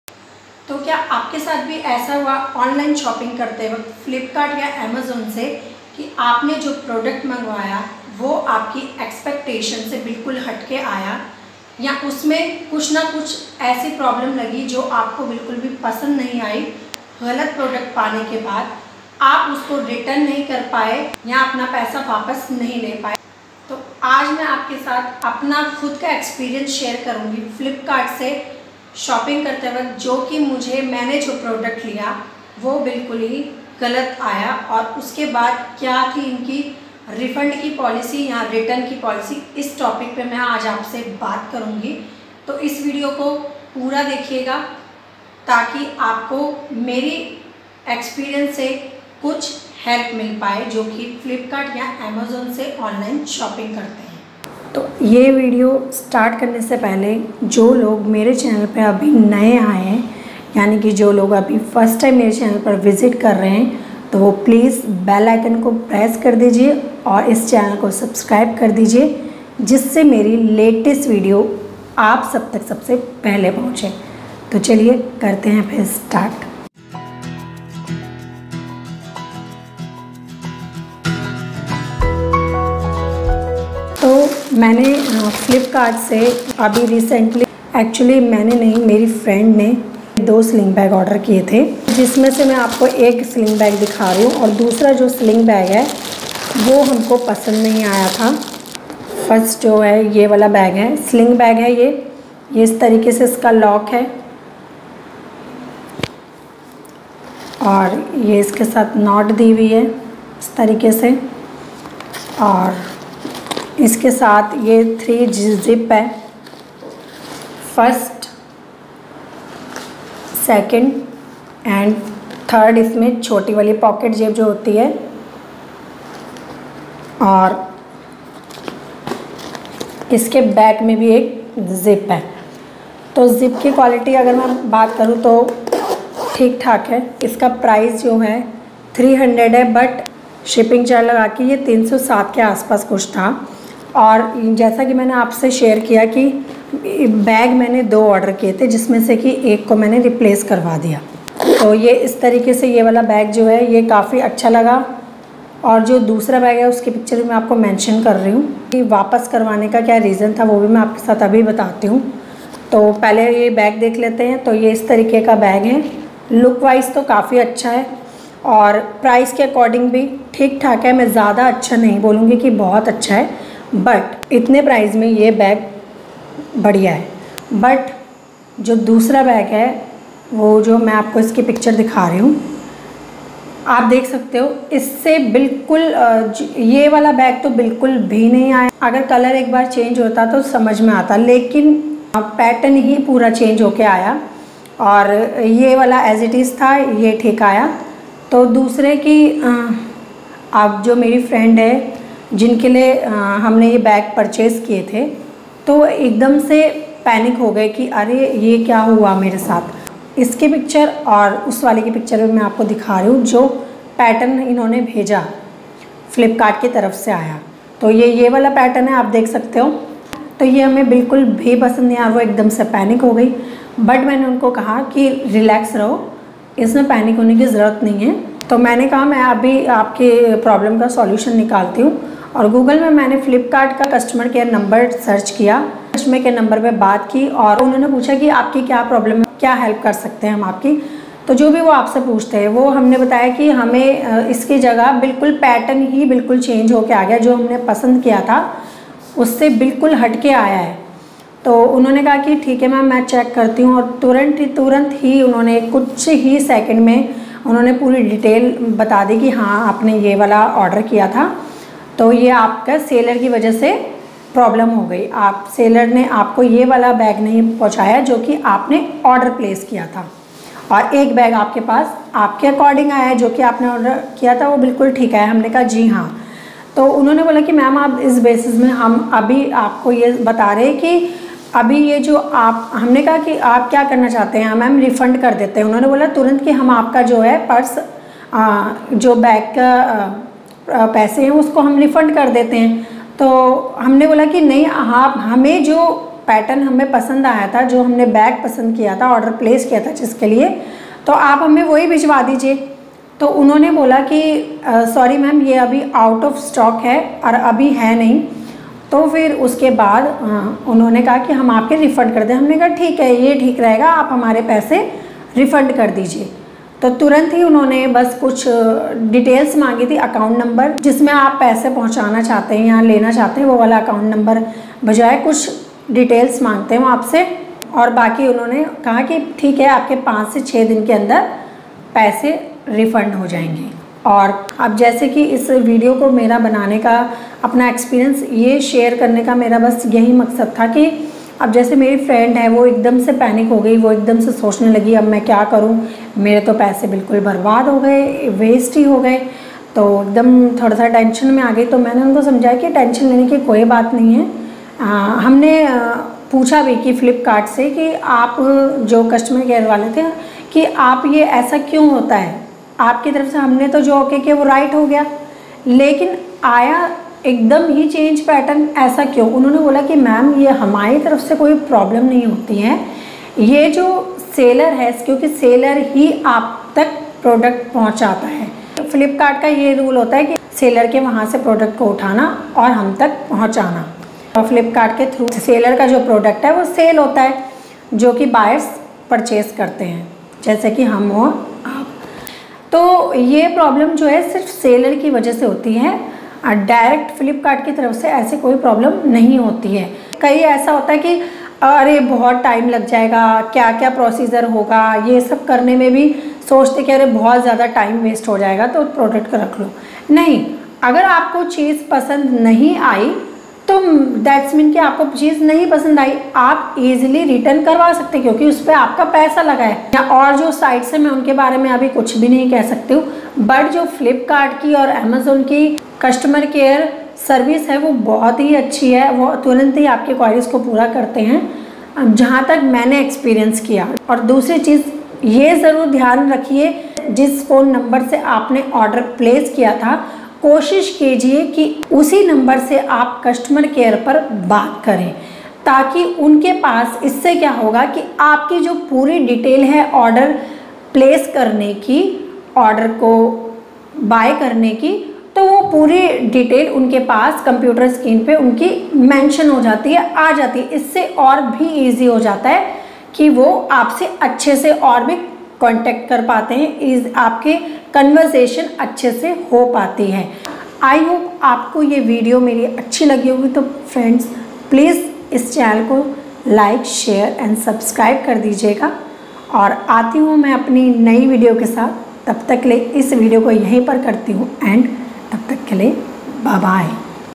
Flipkart और Amazon से सामान गलत आने पर क्या करें और कैसे पैसे वापस लें? Sharing my experience
I purchased sling bag from flipkart but the product was deferent from my order. How I refund back the product and got my money back.

This is my personal experience shop with flipkart.

Amazon or flipkart se Online shopping karke galat product aane par wapas kaise kare or apna paisa kaise wapas le.

In my vlogs you will see daily routine, kitchen tour,  food vlog, tour vlogs,  uttrakhand vlogs,

My Stand

My Mic

My Gorrila Selfie Stick